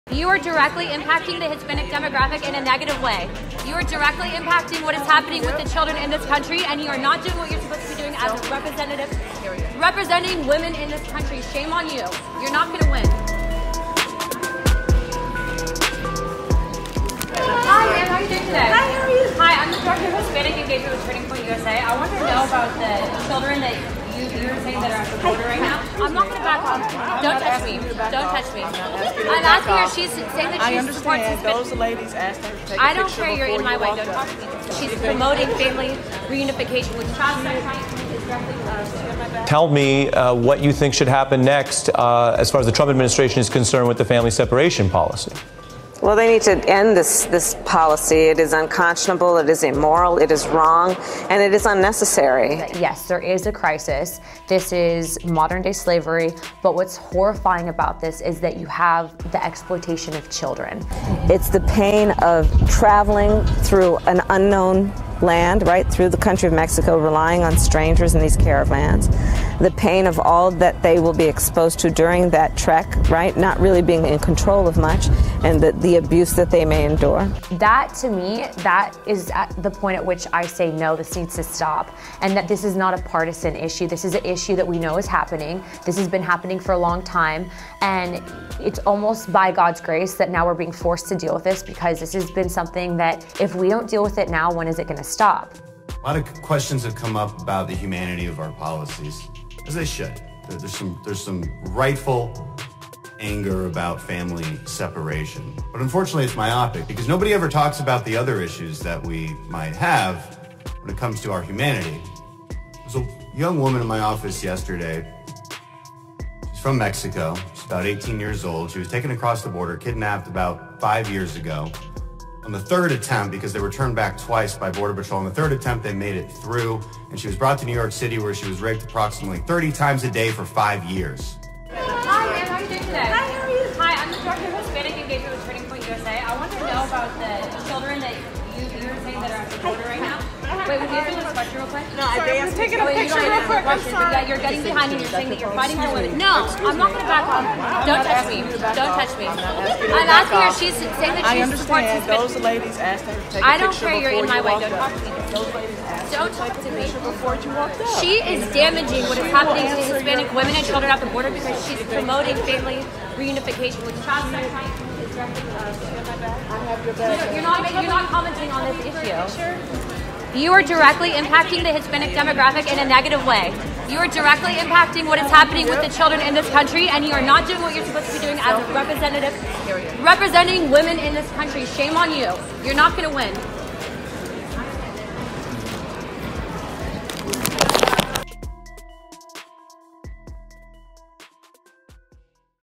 Kamala Harris Challenged By A Conservative Latina On Immigration
Senator Kamala Harris was confronted by Anna Paulina, a conservative Latina working with TPUSA, about sex trafficking through illegal immigration